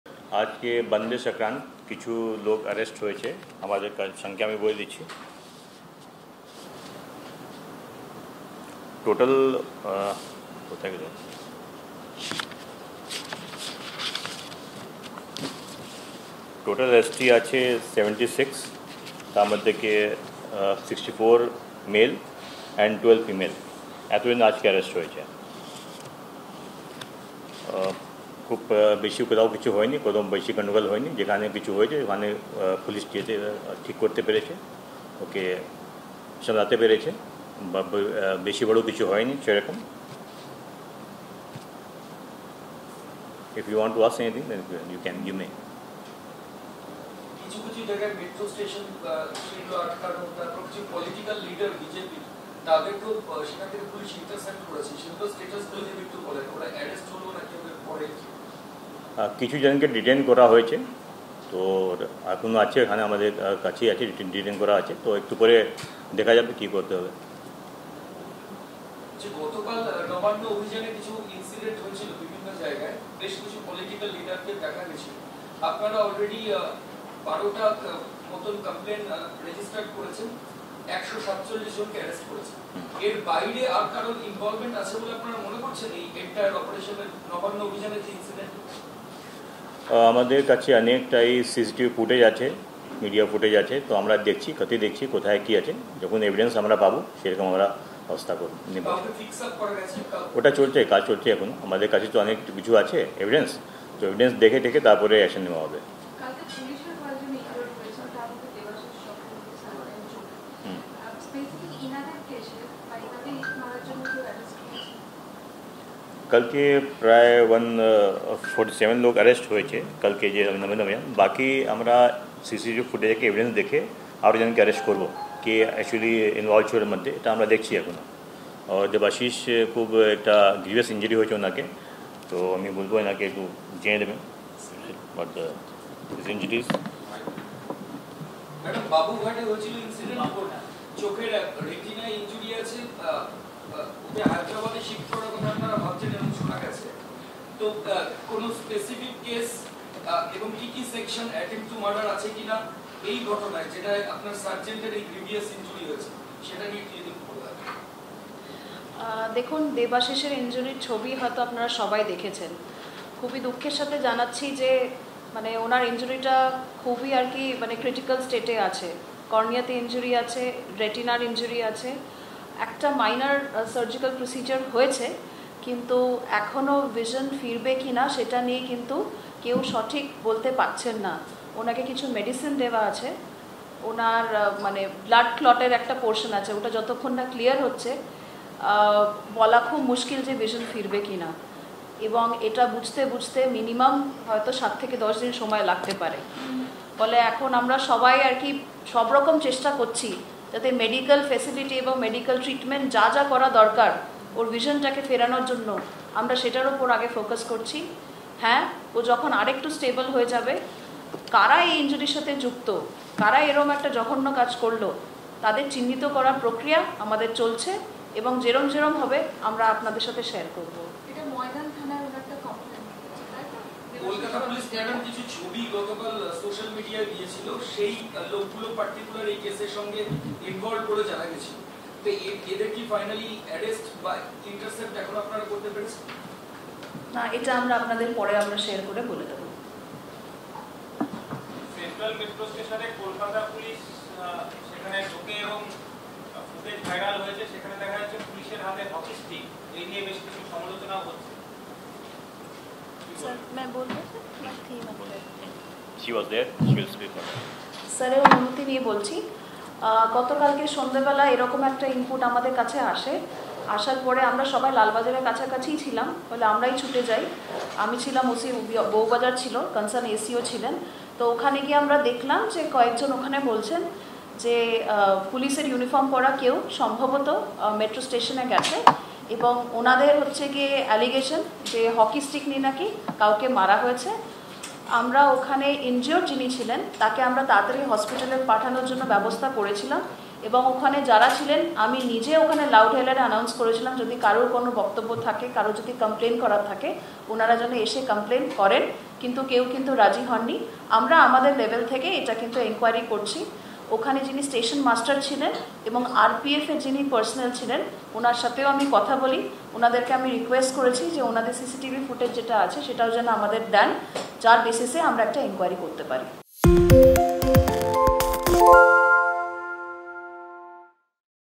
Bangla Bandh: আজকের বনধে গ্রেফতারির সংখ্যা নিয়ে কী বলল পুলিশ ?
ABP Ananda Live: আজকের বনধে কিছু লোক গ্রেফতার হয়েছে। মোট গ্রেফতার হয়েছে ৭৬ জন। তারমধ্যে ৬৪ জন পুরুষ এবং ১২ জন মহিলা আজকে গ্রেফতার হয়েছে।  কোথাও বেশি গণ্ডগোল হয়নি।  যেখানে খুচরো গণ্ডগোল হয়েছে সেখানে পুলিশ গিয়ে সমস্যার সমাধান করেছে। কালকের নবান্ন অভিযানে কিছু ফুটেজ আমাদের কাছে রয়েছে।  আমরা খতিয়ে দেখছি , দেখার পর ব্যবস্থা নেব।, মন্তব্য পুলিশের ।

 / abpanandatv

আপনার পছন্দের খবর আর আপডেট পাবেন আপনার পছন্দের চ্যাটিং প্ল্যাটফর্ম হোয়াটস অ্যাপেও। যুক্ত হোন ABP Ananda হোয়াটস অ্যাপ চ্যানেলে।